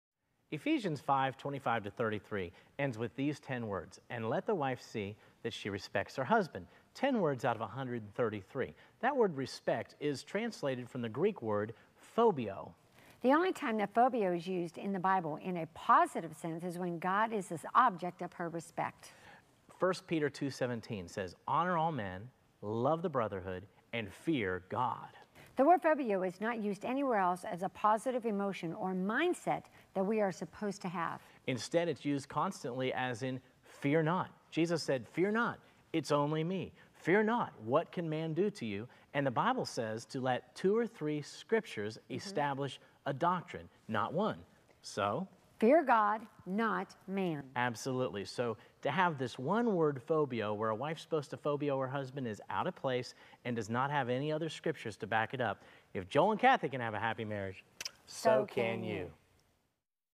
Love and Respect #2 The Great Truth
Joel and Kathy Davisson share another one minute marriage counseling type message. Part 2 on "Respect". Does the Bible really command a wife to "respect" her husband? What does the Greek word mean? What type of respect is correct?